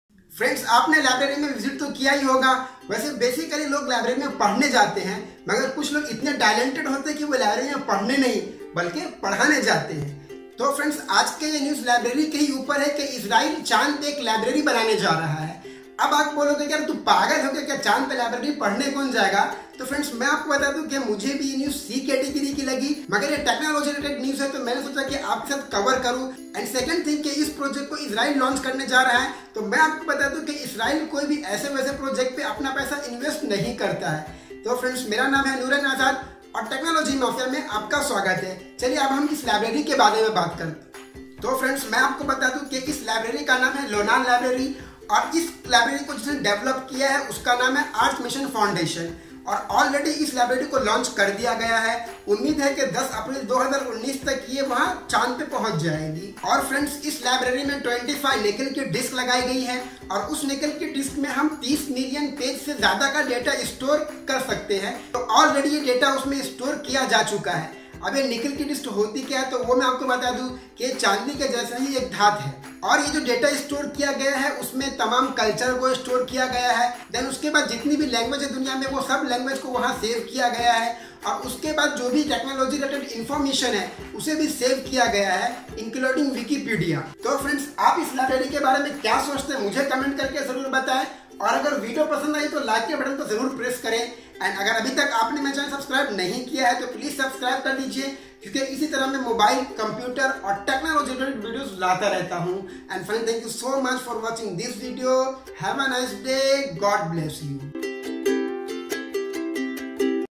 Library on Moon || Lunar library || Israel going to launch Library on Moon (Hindi)
Israel going to make library on moon. This library name is “Lunar Library”. In this video I try to explain each and everything about “Lunar library” like?
1)When Lunar library launched?
2)Need of this Lunar library?
3)How much data are store in this Lunar library? and much more.

                                    TECHNOLOGY MAFIA